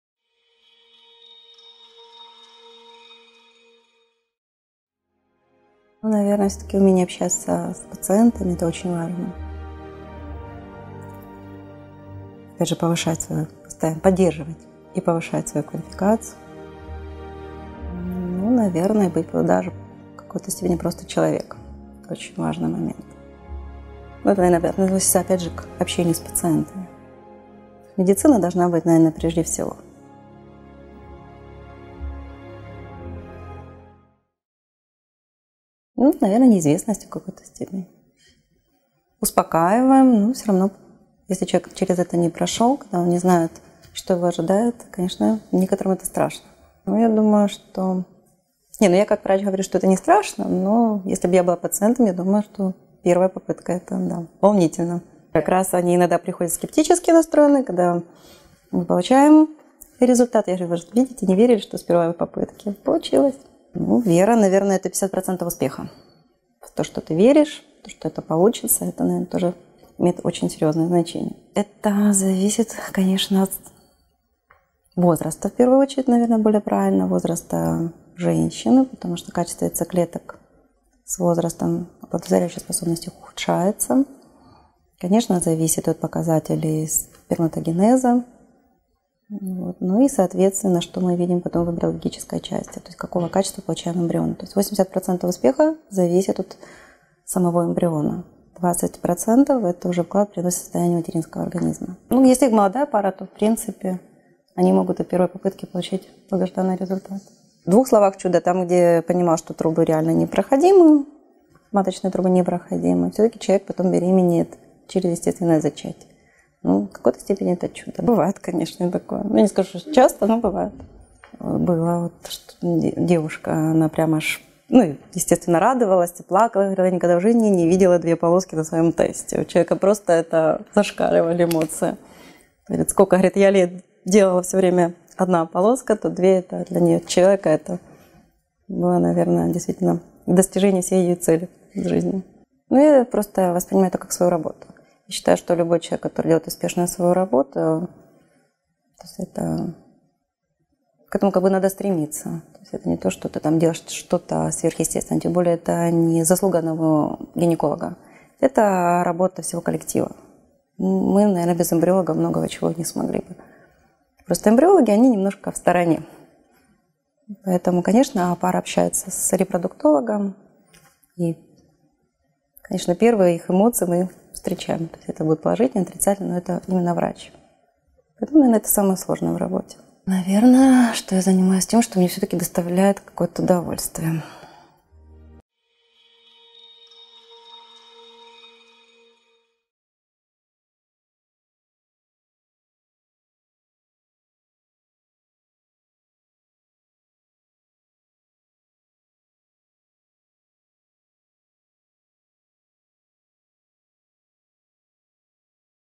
Feskov HRG: reprodutologista Natalya Chumakova sobre a FIV, curiosidades e expectativas
Ginecologista, médica de ultrassom, especialista no tratamento da infertilidade FHRG Natalya Chumakova contou quais sentimentos e medos as clientes têm na véspera da FIV, quais fatores aumentam a probabilidade de uma FIV bem-sucedida e do nascimento de uma criança saudável, fala sobre casos incríveis e seu próprio papel num grande processo médico. Fala sobre fatores que definem o resultado da FIV ― mais detalhes nesta entrevista.

Primeira questão ― sobre as emoções de clientes antes de começar a trabalhar. Claro, o medo da ignorância é muito forte. Os médicos neste momento acalmam e encorajam, mas Natalya admite que entende bem o paciente e no seu lugar sentiria as mesmas emoções.

Mas como é agradável informar o resultado a pacientes inicialmente céticos! Especialmente se a primeira tentativa for bem-sucedida.

A doutora está convencida de que a fé e uma atitude positiva de pelo menos 50% determinam o sucesso de qualquer procedimento.

A segunda questão preocupa todos os nossos potenciais pacientes. Fazem esta pergunta nas negociações, antes do início da cooperação: “do que depende o sucesso da FIV?”. Depende em primeiro lugar da idade da mulher, pois os óvulos perdem a capacidade de fertilizar com o tempo. Mas a qualidade do esperma do futuro pai não é menos importante. A qualidade final do embrião depende dos biomateriais dos pais, que em 80% determinam o sucesso da FIV e o nascimento de uma criança saudável. Os restantes 20% dependem da contribuição do organismo materno. Frequentemente, os casais jovens têm sucesso logo na primeira tentativa.

Segunda questão ― sobre o sucesso inesperado, apesar de, não por causa de. A doutora se lembra de uma jovem cujas trompas de falópio eram intransponíveis, mas depois de vários anos de tentativas infrutíferas, ela conseguiu engravidar naturalmente. Não havia limite para felicidade e emoções. Isso acontece quando o objetivo principal da vida é alcançado.

Terceira questão ― sobre atitude para com o trabalho. Natalia Chumakova não vê proeza e heroísmo no desempenho de suas funções profissionais. Acredita que todos devem se esforçar para isso de uma forma natural. Enfatiza que os sucessos ou fracassos da FIV e o nascimento de filhos saudáveis entre os clientes são mérito não de um médico que assiste, mas de toda a equipa. Reconhece os méritos dos embriologistas, cuja contribuição é indevidamente silenciosa. O prazer de fazer o trabalho usual, mas querido, é o principal motivo do médico para continuar seu trabalho.

Feskov Human Reproduction Group – é a felicidade dos pais para aqueles que perderam a esperança!